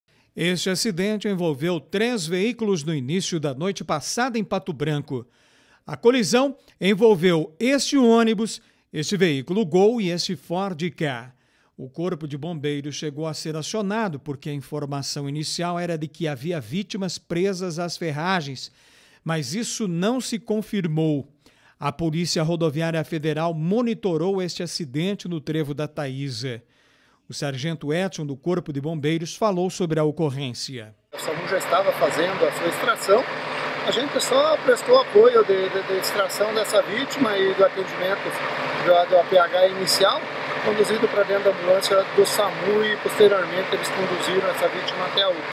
Colisão entre três veículos em Pato Branco
Acidente: O choque foi registrado no início da noite de ontem (13).